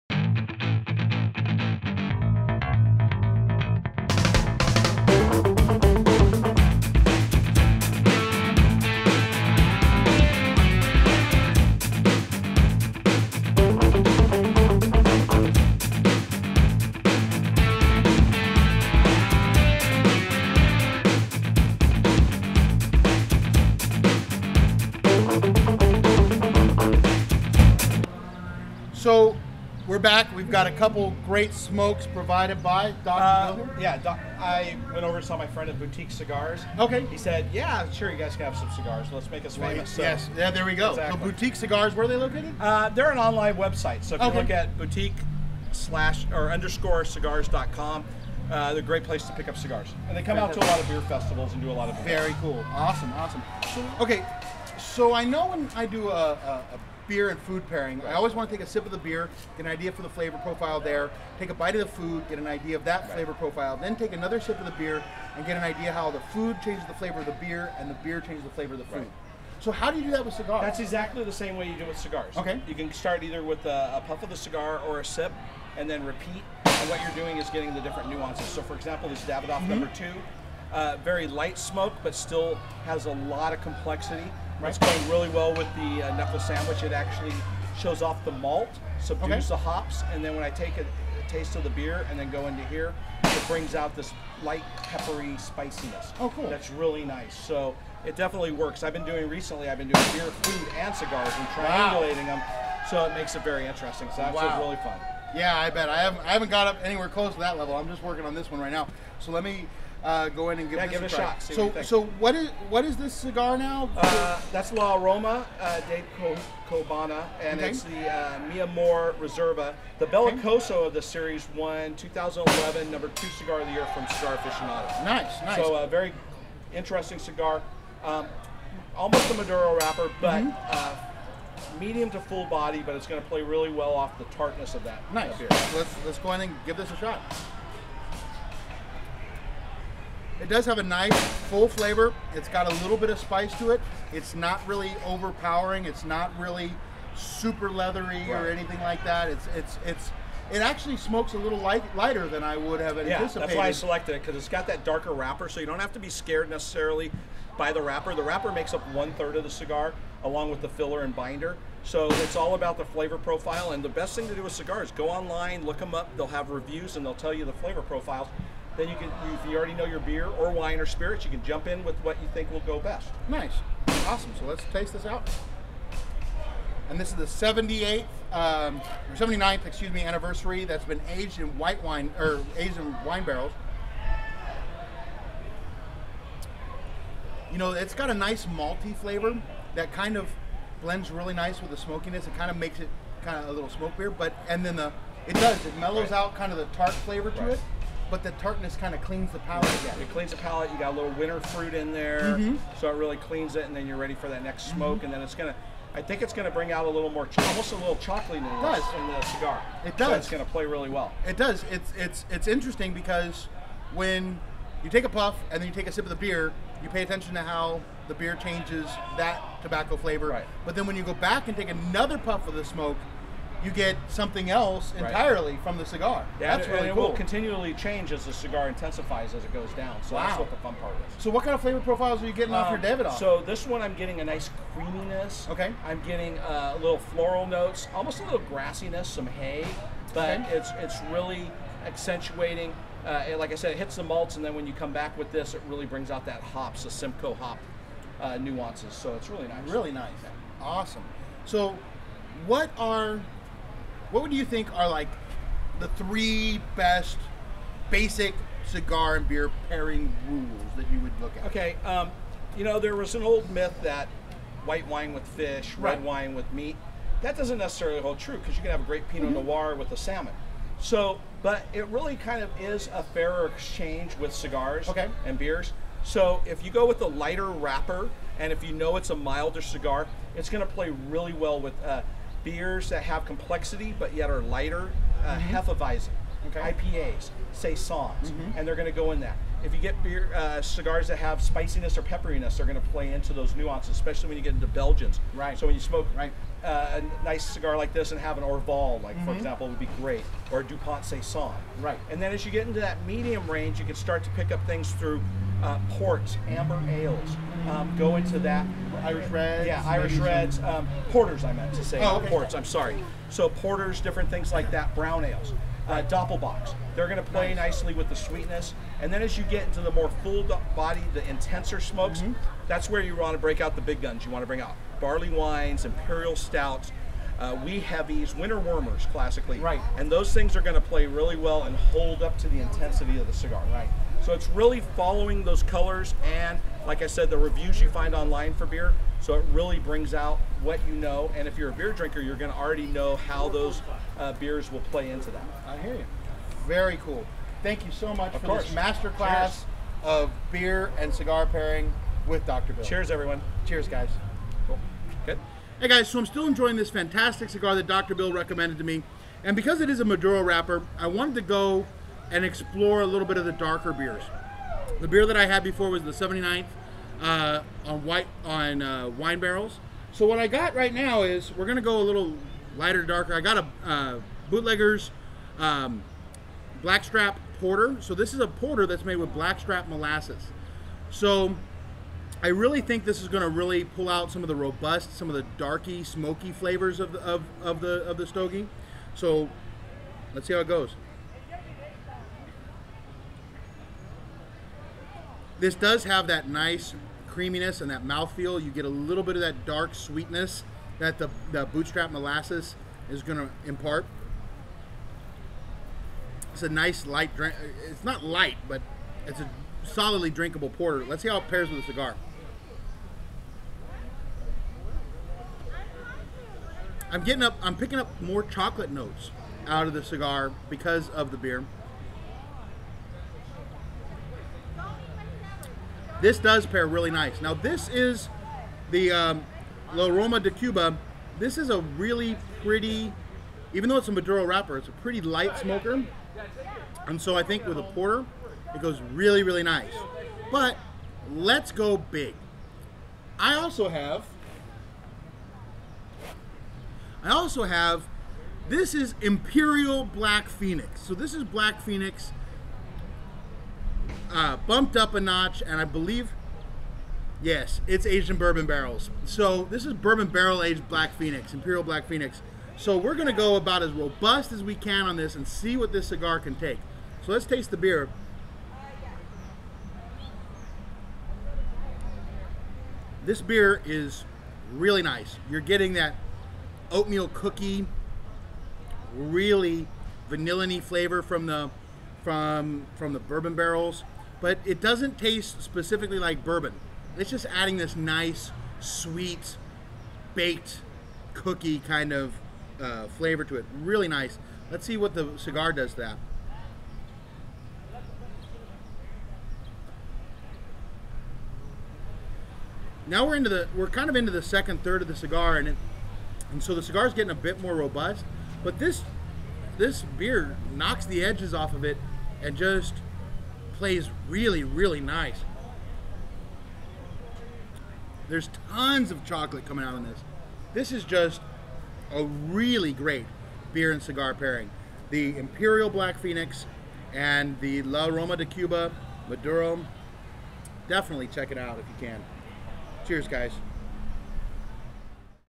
Cigar and Beer Pairing Master Class with Dr. Bill Sysak!
We caught up with Dr. Bill Sysak from stone brewing at the Bootleggers 5th anniversary. He was kind enough to give us some tips on how to pair craft beers with your cigars! Cheers.